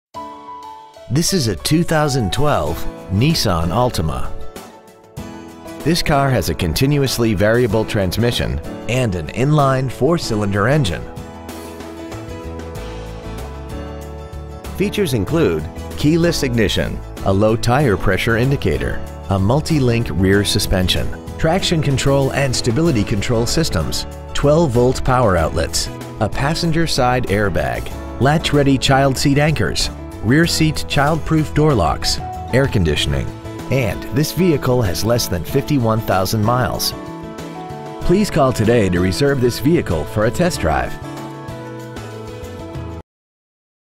Pre-Owned 2012 Nissan Altima Houston Texas 77034
We have been honored to serve the Houston Texas area , we promise that your experience at our dealership will exceed your expectations ! Year : 2012 Make : Nissan Model : Altima Engine : Gas I4 2.5L/ Trans . : Variable Exterior : Super Black Miles : 50,004 Interior : Black Champion Toyota Gulf Freeway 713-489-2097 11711 Gulf Freeway Houston , Texas 77034